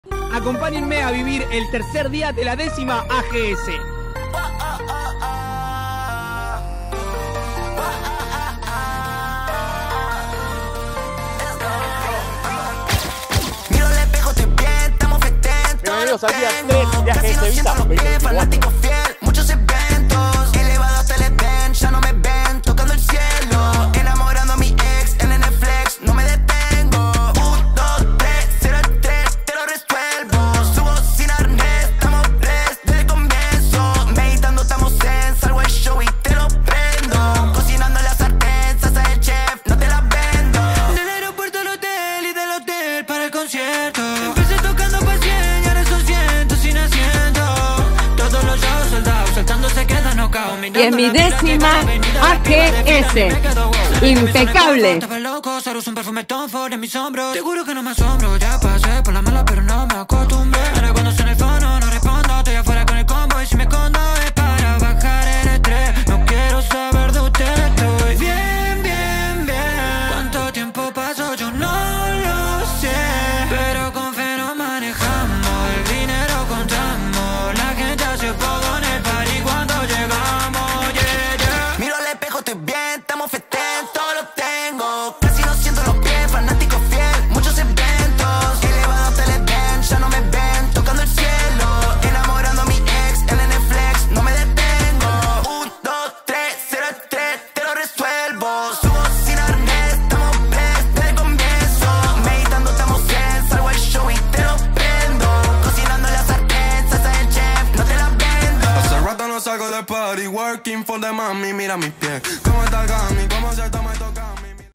Recap AGSVisa día 3
Último día de LaDecima edición de AGSVisa, un placer volvernos a encontrar y recibir todo su amor, esperamos que les haya encantado, y que nos volvamos a ver el año que viene!!